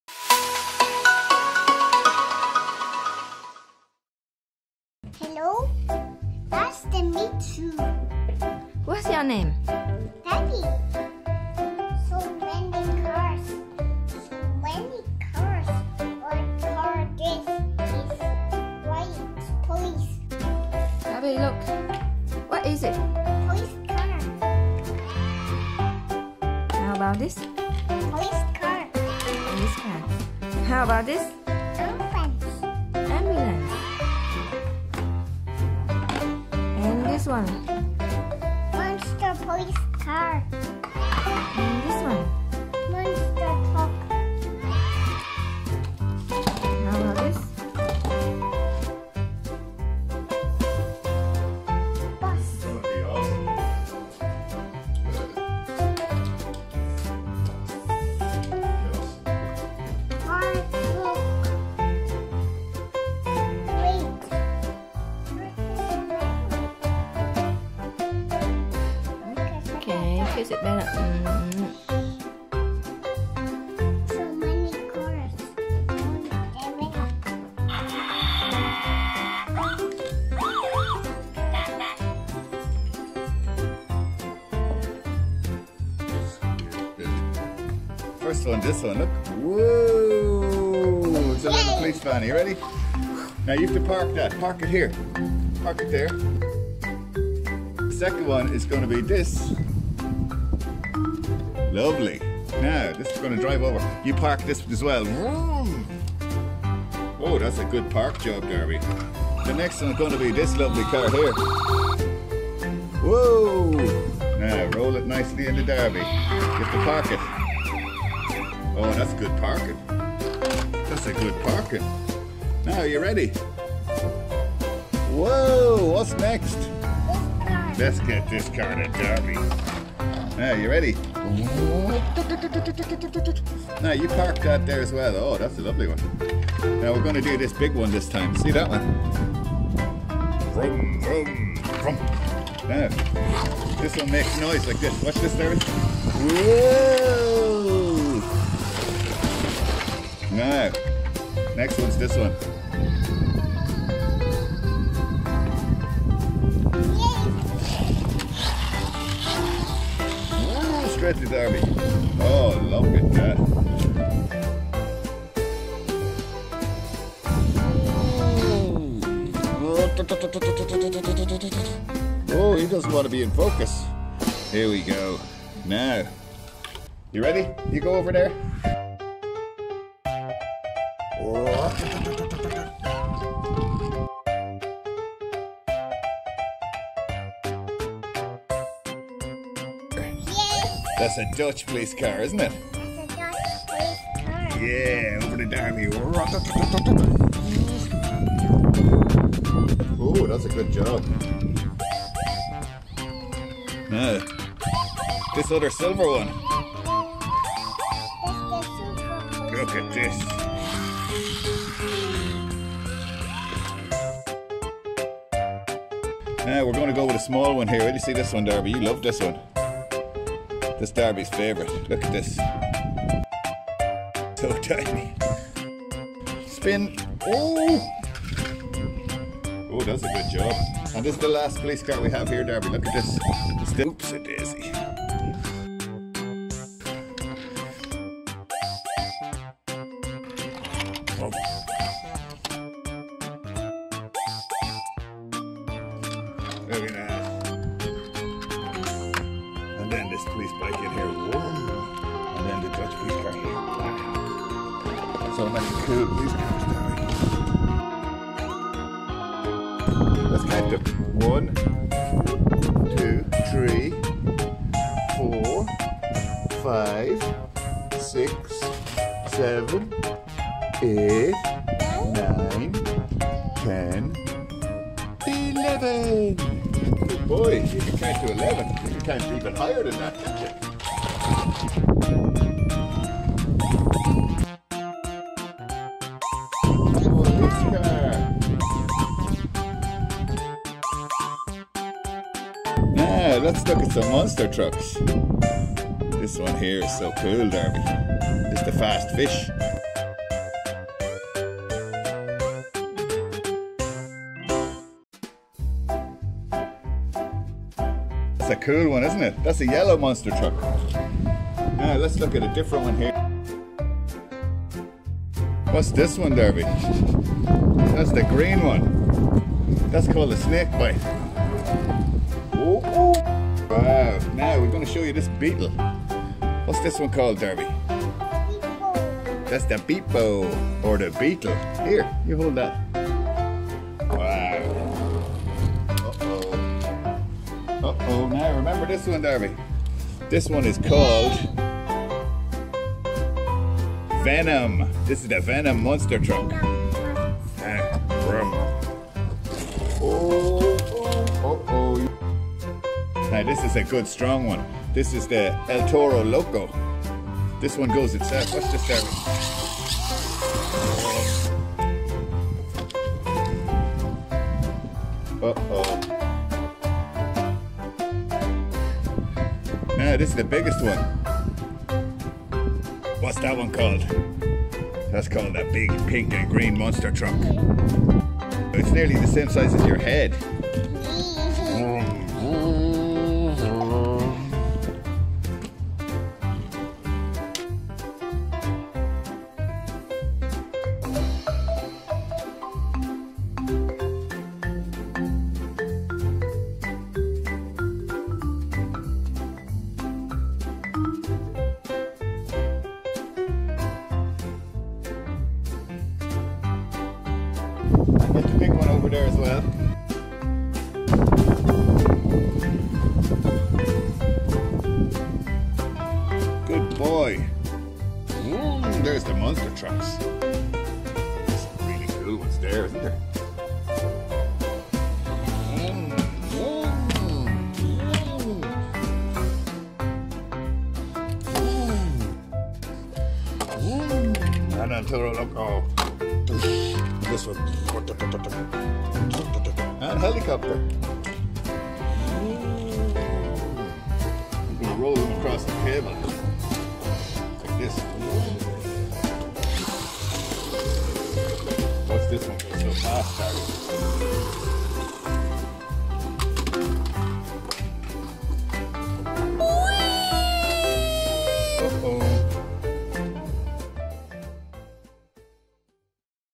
Baby playing with toy cars and monster trucks
Baby playing with toy cars and monster trucks, so much fun! Let's check it out😍😍😍

Darby chơi với mấy xe đồ chơi vui dễ sợ😆, có cả phụ đề tiếng Anh cho bạn nào cần nha hihi 😍
More Unboxing Cool Toy Cars: